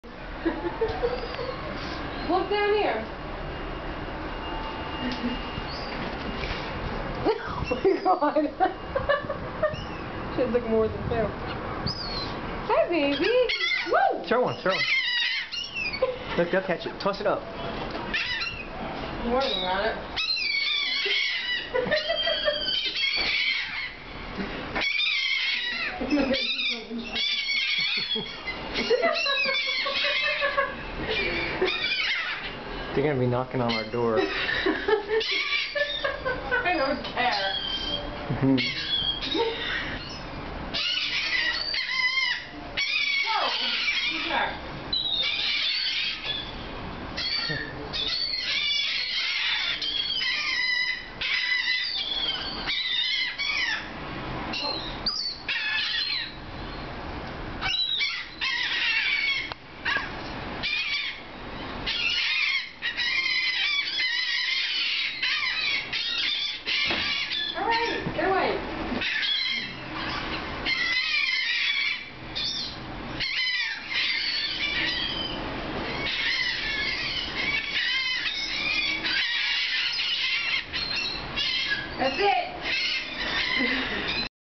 Nassau Bahamas - Maggie Feeding Seagulls from Room Balcony docked in Nassau
Balcony in our room on Cruise Ship Norwegian Jewel.  Ship is docked in Nassau Bahamas January 2012.